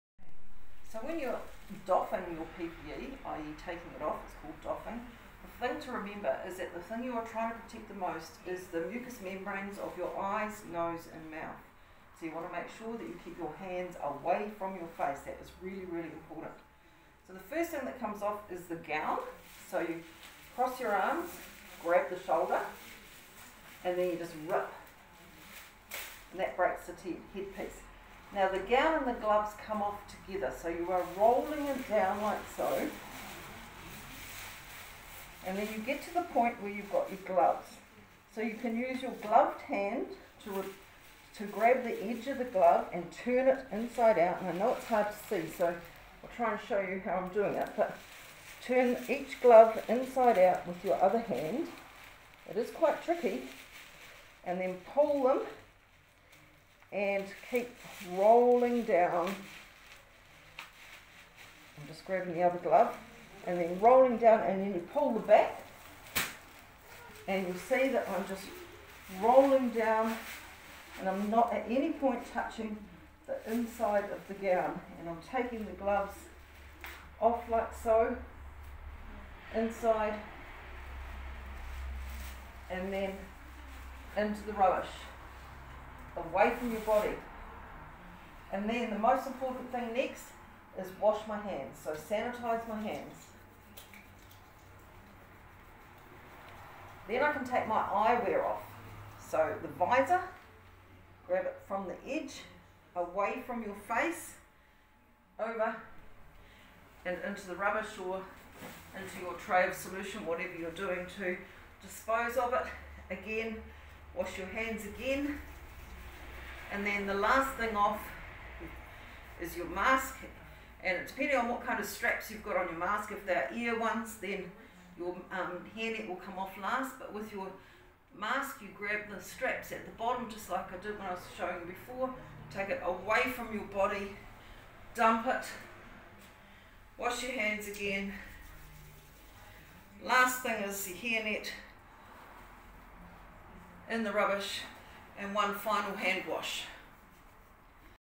Doffing (removing) PPE
This video explains how to safely remove PPE (Personal Protective Equipment)